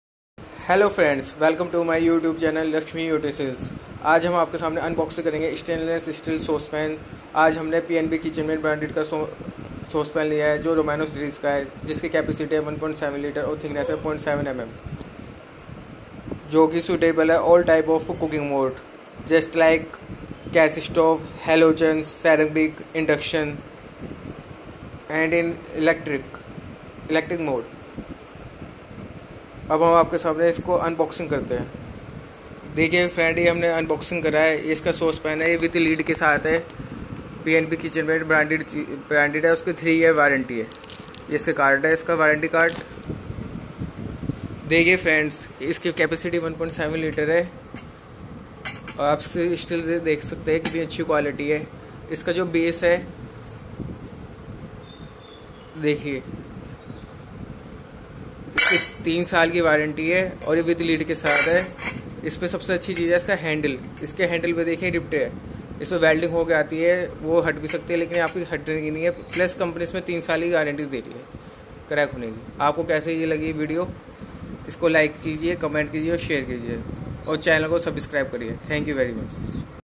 Unboxing Stainless Steel Saucepan
Unboxing & Review of Pnb Kitchenmate Stainless Steel Saucepan 1.7 Litre with Steel lid

It can be used on all Domestic Gas
Induction, Halogen, Electric and Ceramic